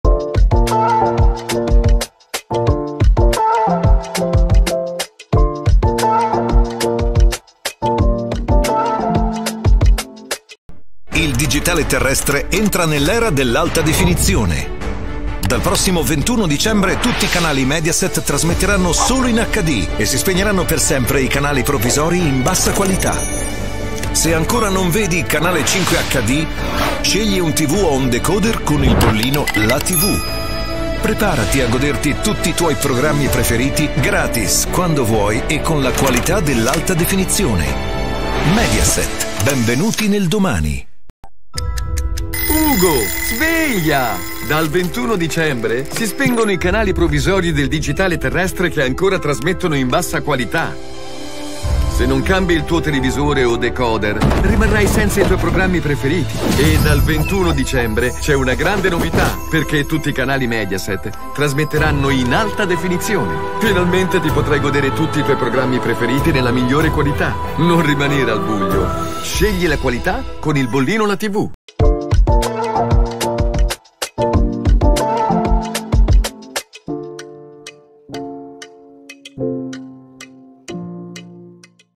2 spot Mediaset: canali trasmessi solo in HD | Novembre 2022
A partire dal 21 dicembre sul digitale terrestre i canali Mediaset saranno trasmessi solo in alta definizione, verranno quindi spente le versioni in bassa qualità dei canali. Per questo motivo, è necessario dotarsi di un televisore o di un decoder in grado di ricevere i canali già trasmessi in HD, ad esempio Canale 5 HD.

0:00 (Intro)
0:10 Spot: Benvenuti nel domani
0:41 Spot: Bollino "La Tivù"
1:11 (Outro)